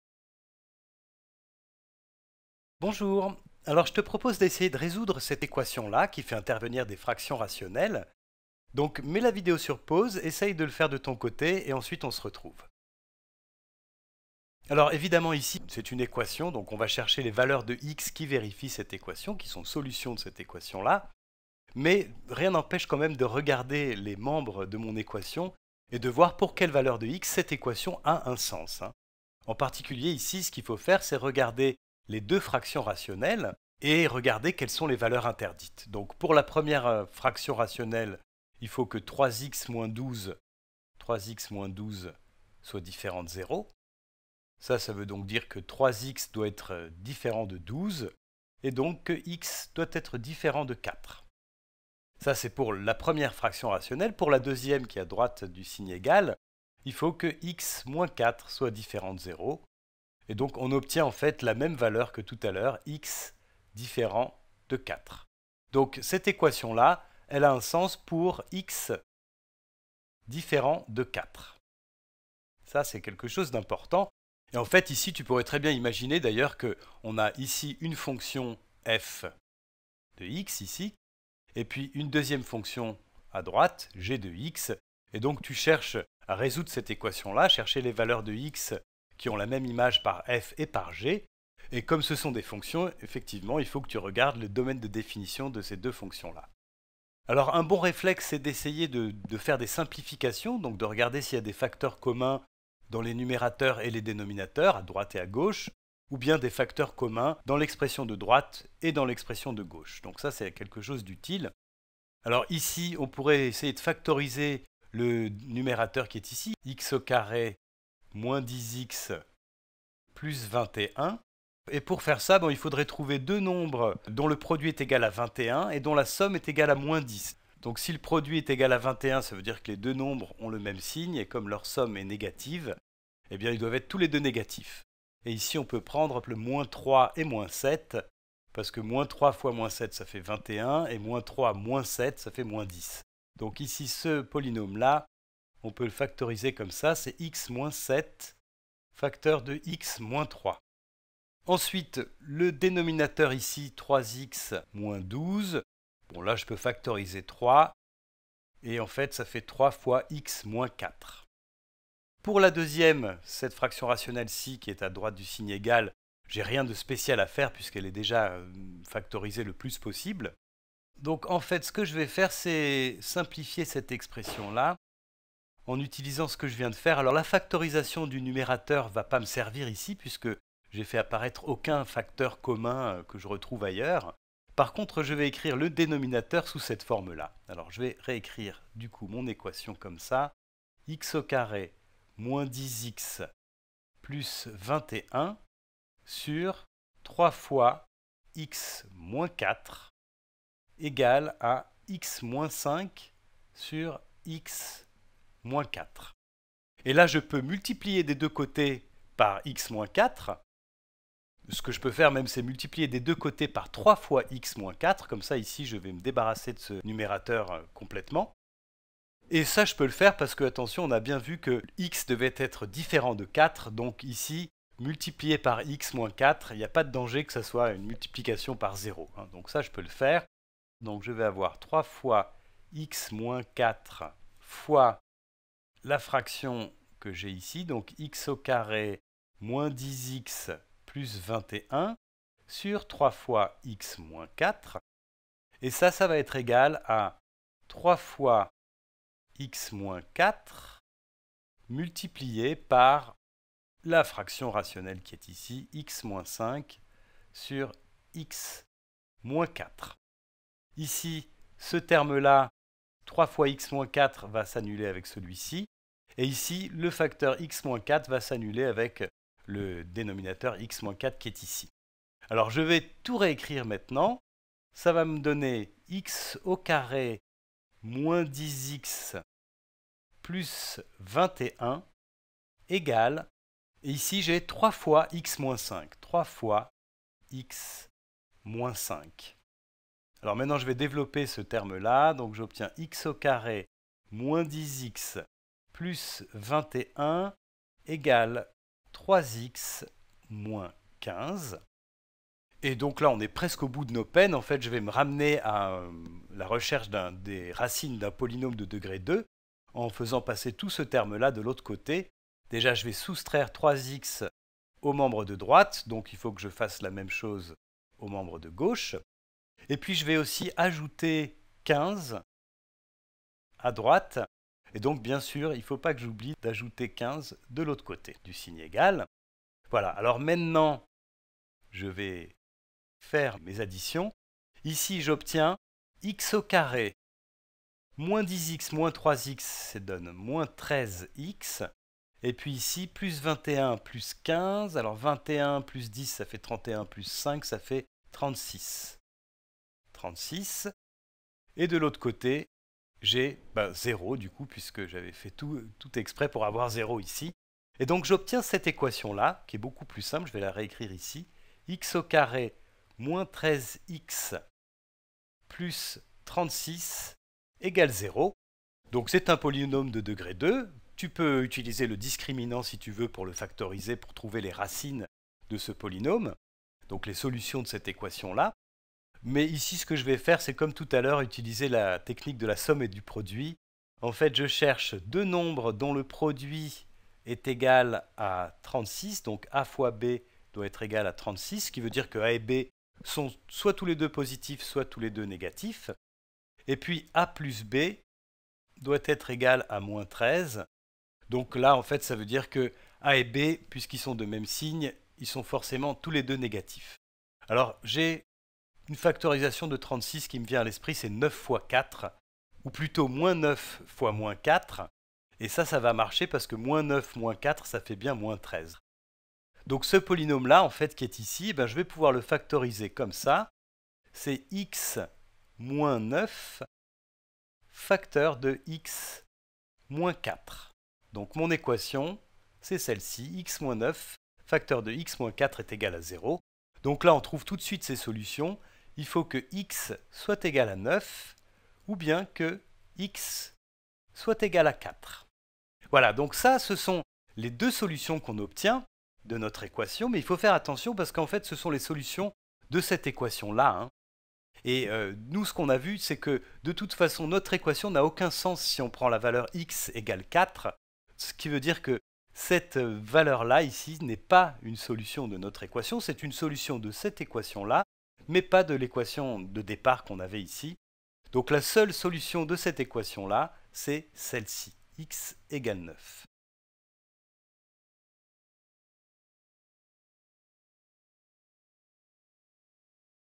Equations avec des fractions rationnelles
Comment résoudre des équations dans lesquelles interviennent des fractions rationnelles.